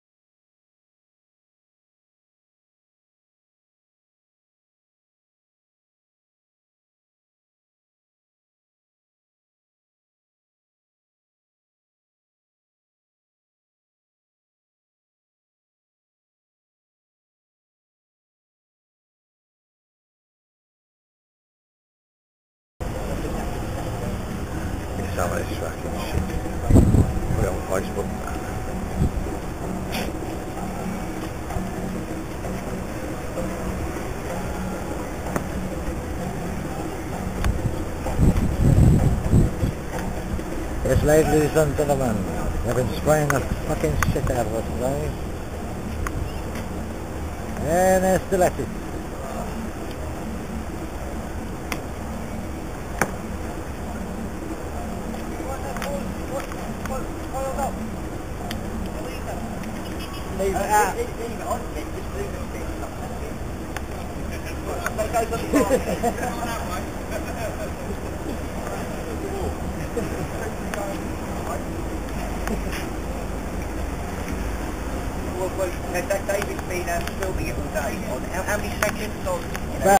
Chemtrails over London Southbank 24.03.11.wmv
I was working over Londons Southbank next to the London eye the other week laying a lawn for a garden we're building. Filming us on timelapse i caught the days chemtrailing. 24/03/11